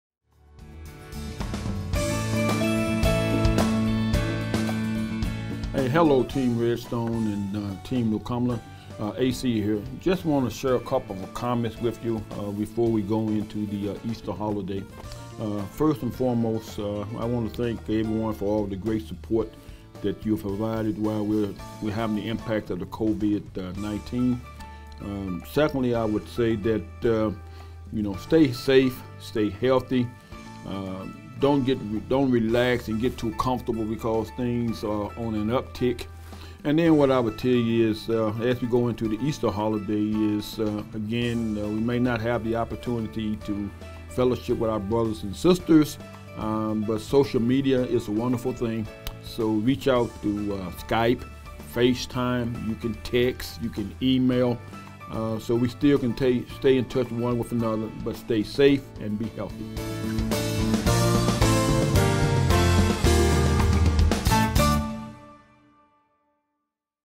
Team USASAC Shout-Out by AC
Antoine Cheatham gives a shout-out for the USASAC workforce.